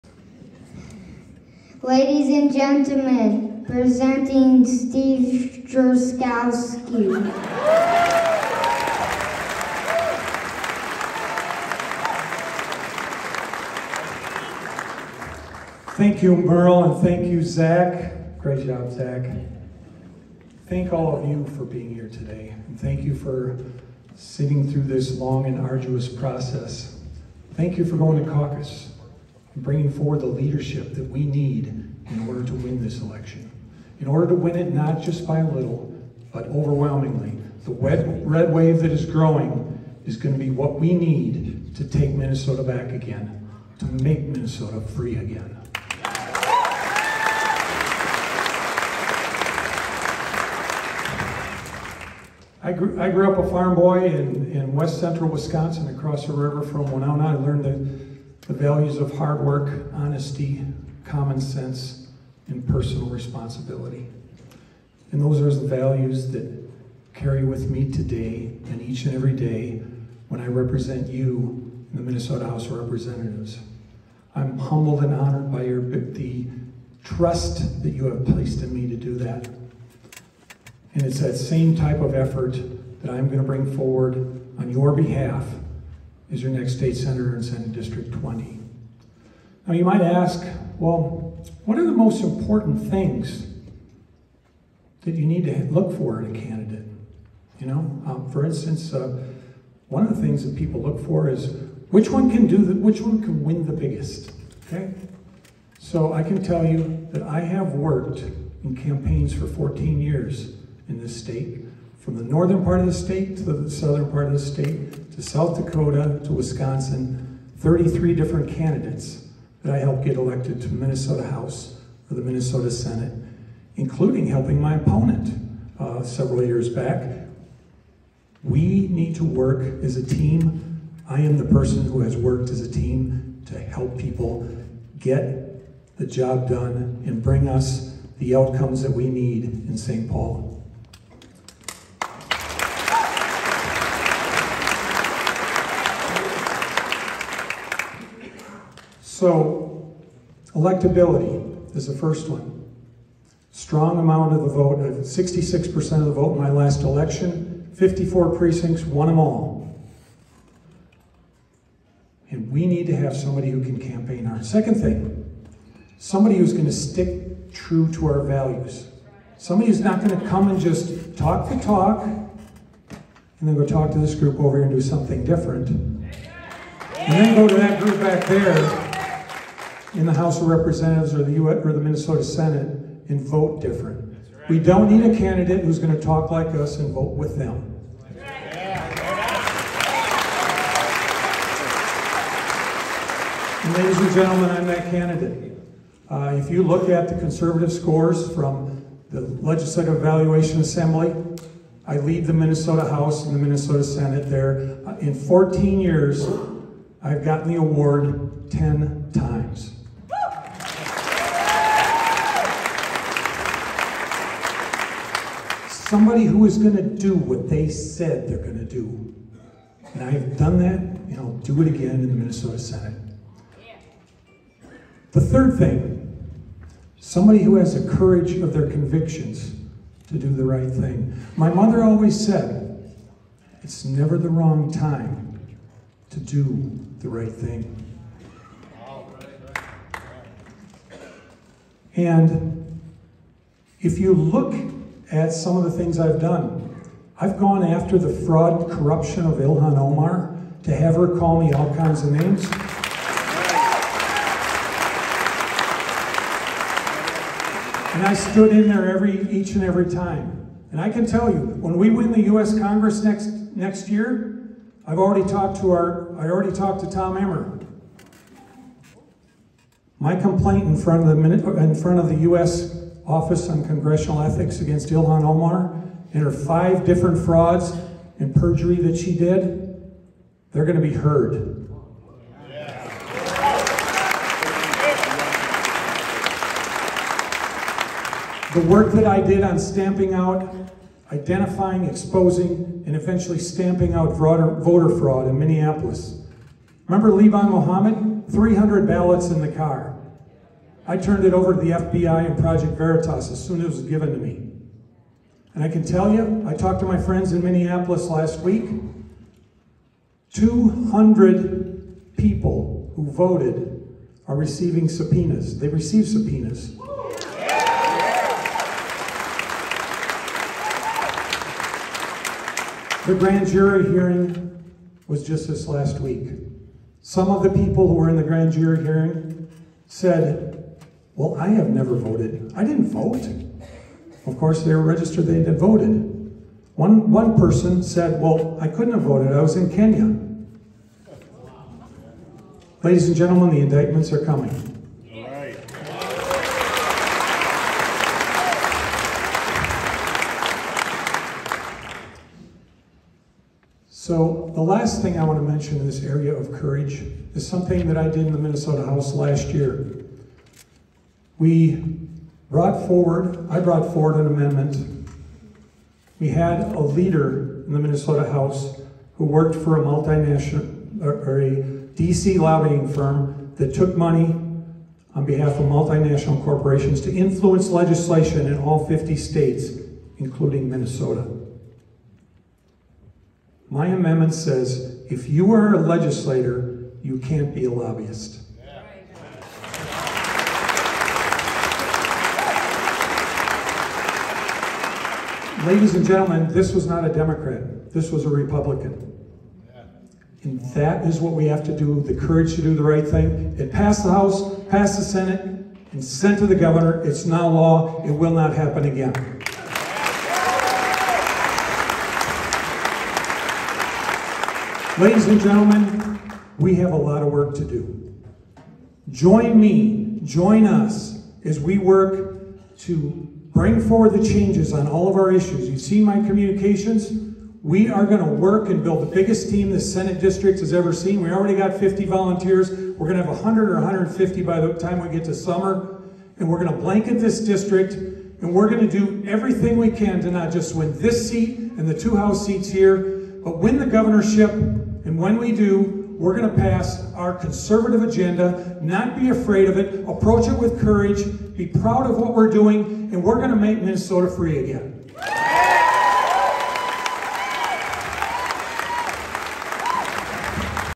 Steve Drazkowski – Nomination Speech
Steve Drazkowski's nomination speech at the GOP Endorsing Convention on Saturday, April 2, 2022.